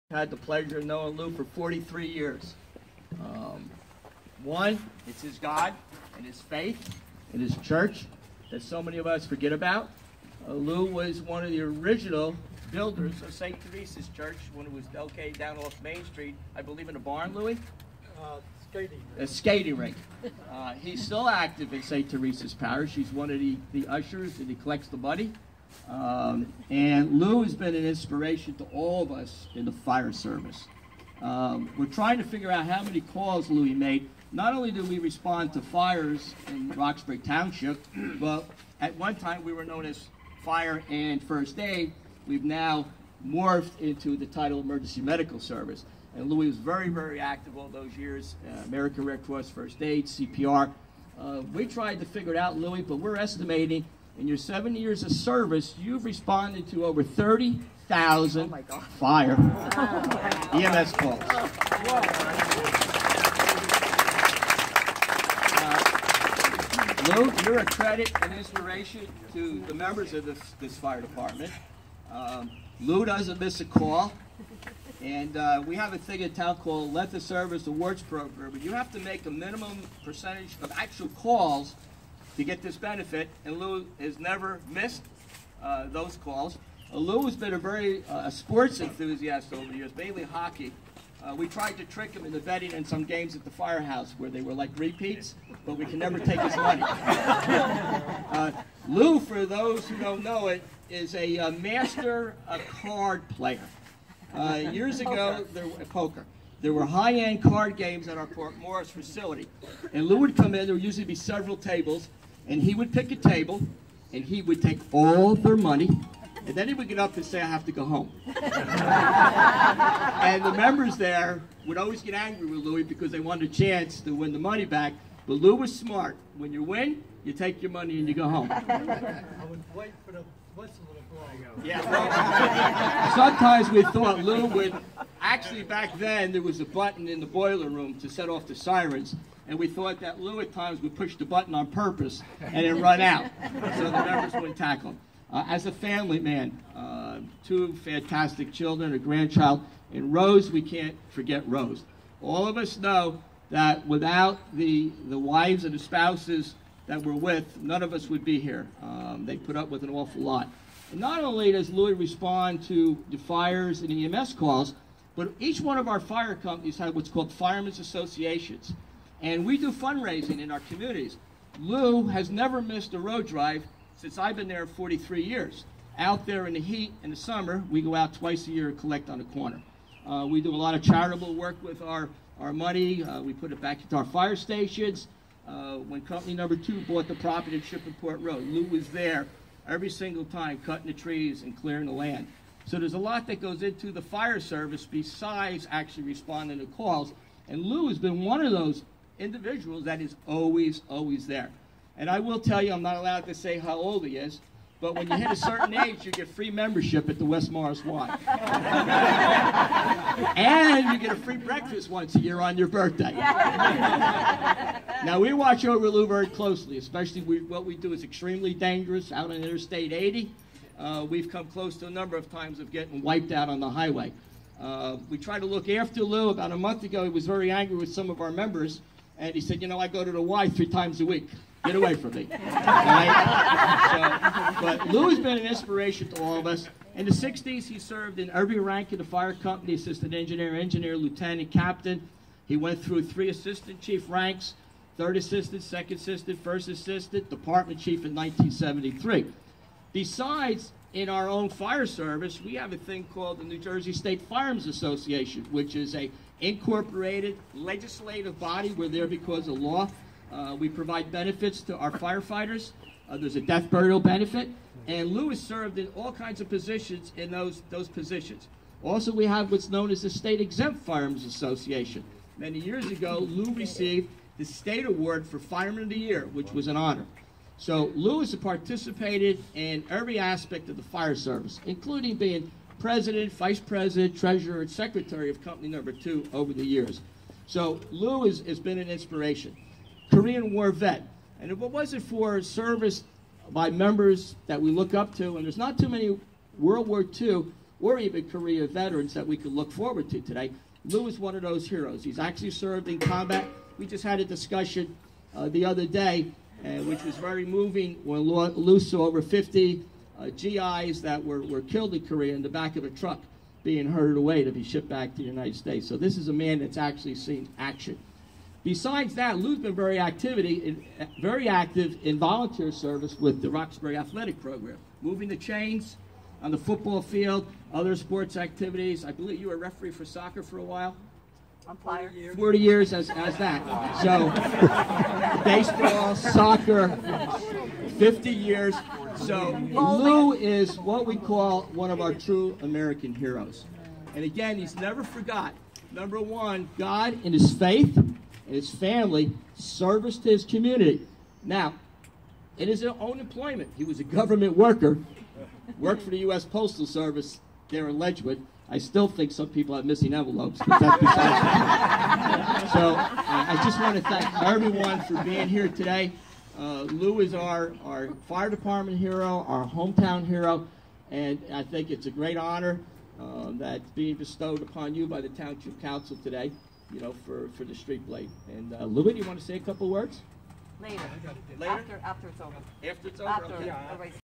Bob Hackett speaks about Lou Bizzari
Longtime Roxbury firefighter Bob Hackett speaks about Roxbury Firefighter Louis Bizzari at ceremony honoring Bizzari for 70 years in the department